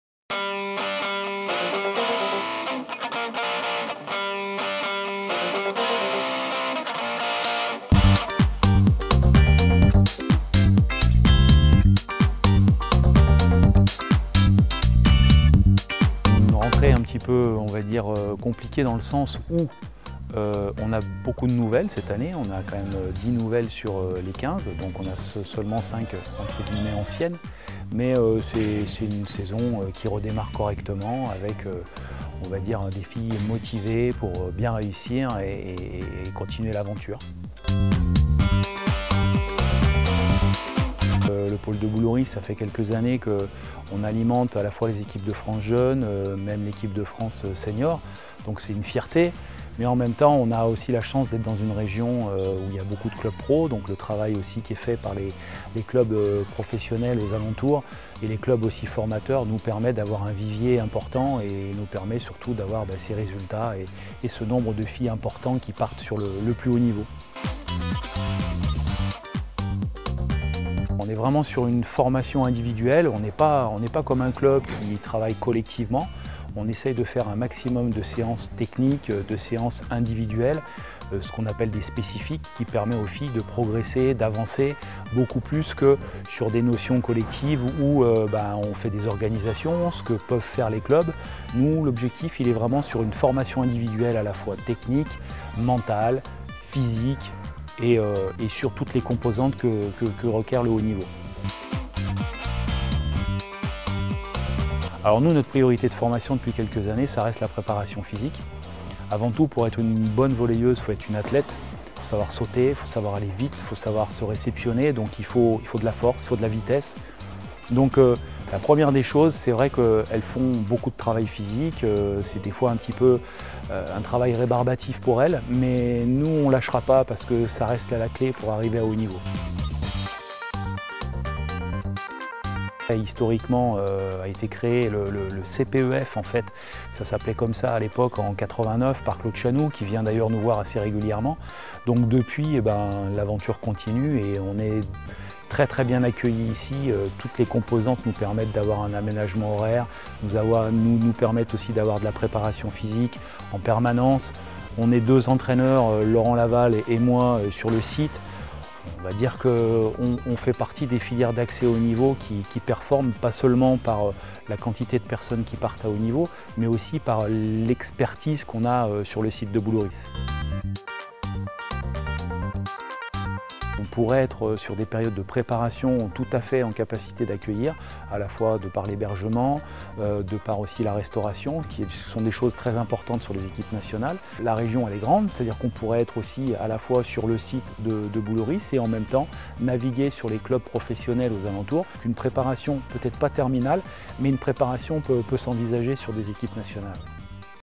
Octobre 2022 / Entretien avec Fabrice VIAL, coordonnateur du pôle espoir de volley-ball du CREPS
Entretien avec Fabrice VIAL, coordonnateur du pôle espoir de volley-ball du CREPS Provence-Alpes-Côte d'Azur (site de Boulouris)